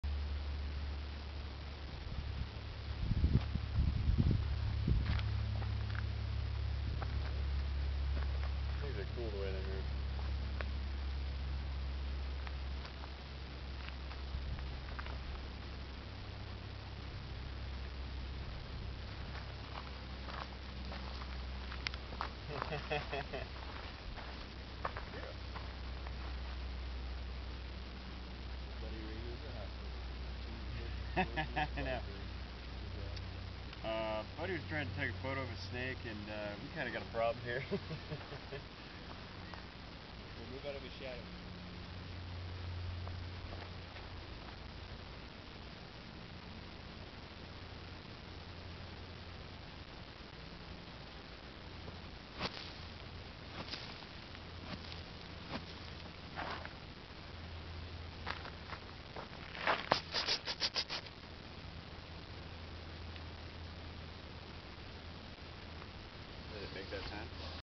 SNAKE of the DESERT!!!
Check out this snake found in the desert of California.  I think its a gopher snake but not sure.  It's about 2.5 to 3 feet long.  Great snake to see on a hot desert day.  For some cool t-shirts check out this